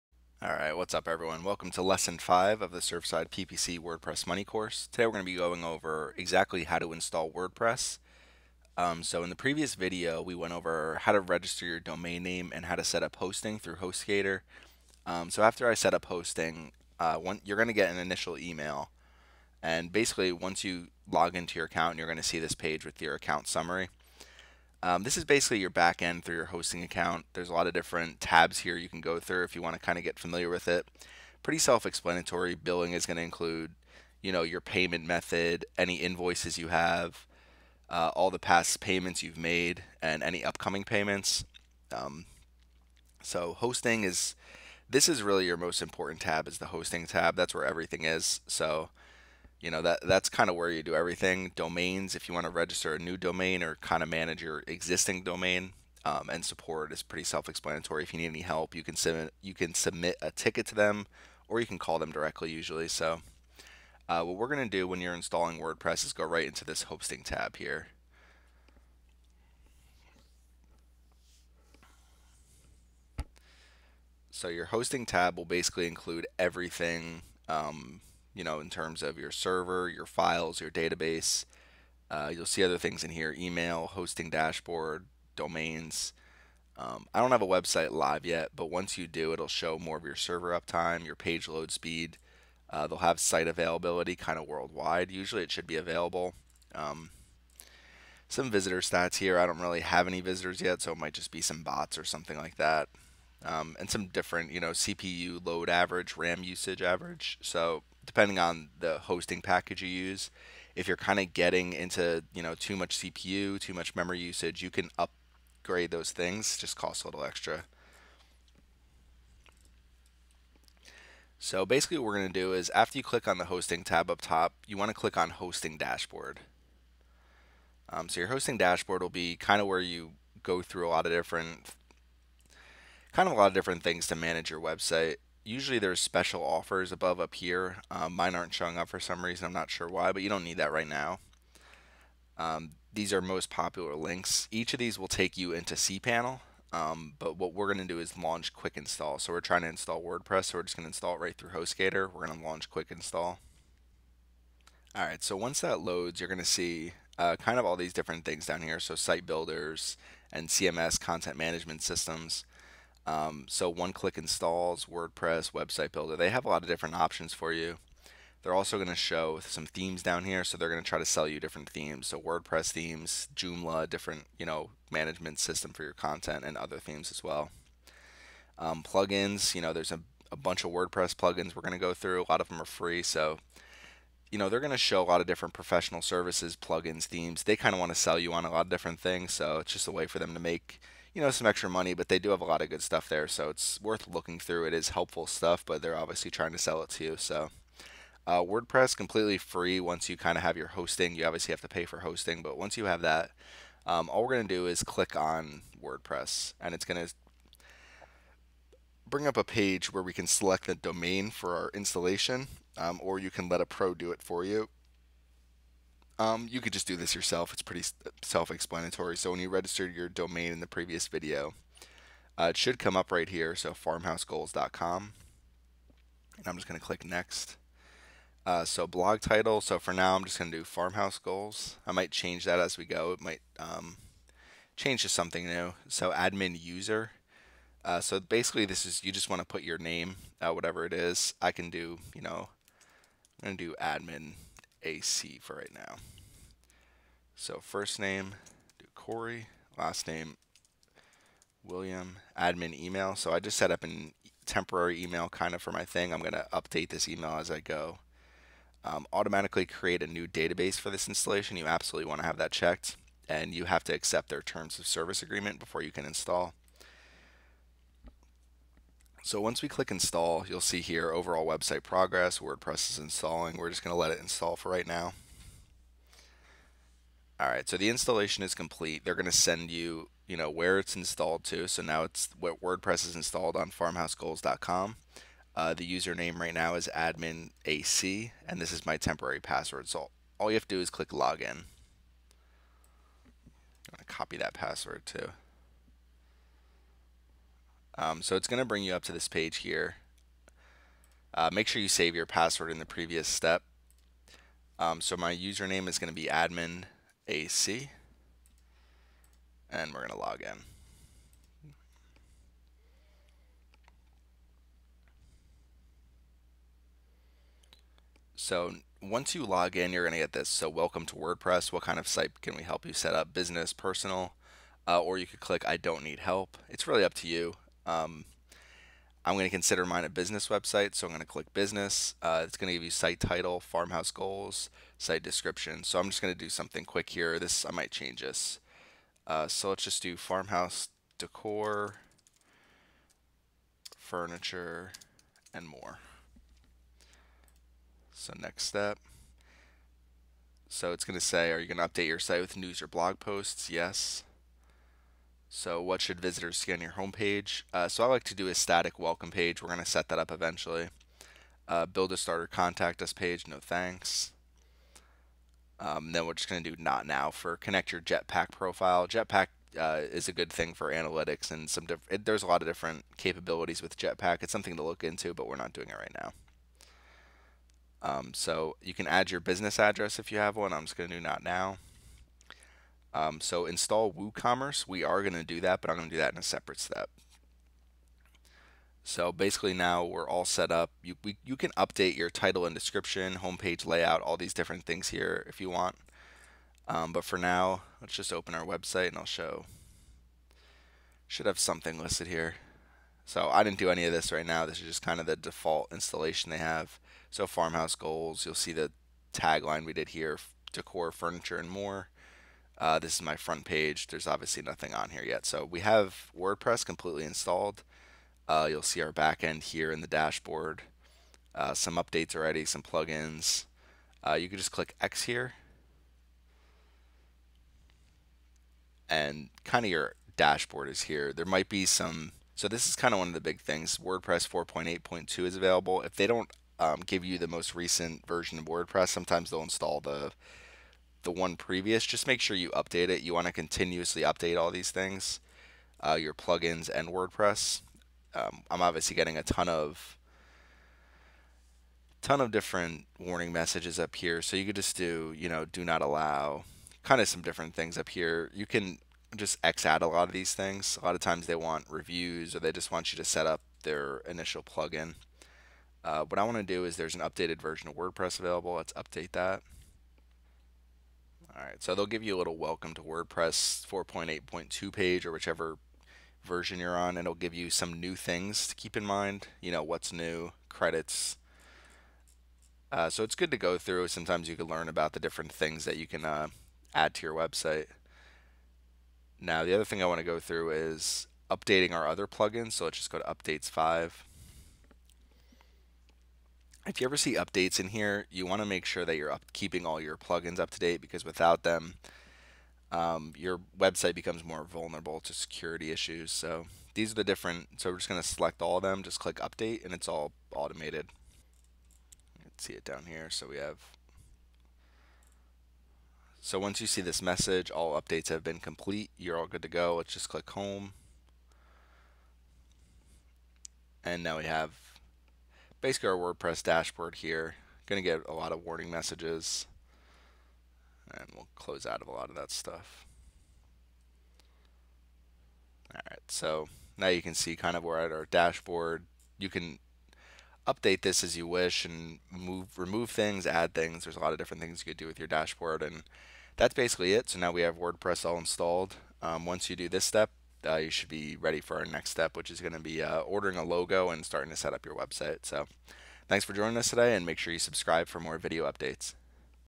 How to Install WordPress on Your Domain - Lesson #5 - Amazon Affiliate Marketing Training
In Lesson 5 of the Surfside PPC Amazon Affiliate Marketing Training Video Series, we go over how to install WordPress for your domain. You will have hosting, a domain name, and now WordPress installed. You absolutely need to install WordPress to continue with this video tutorial and you will love using this amazing content management system (CMS).

We hosted our website through HostGator and registered our domain in the previous step. There are different ways to Install WordPress but we go over a way to go through HostGator and use Quick Install with one-click.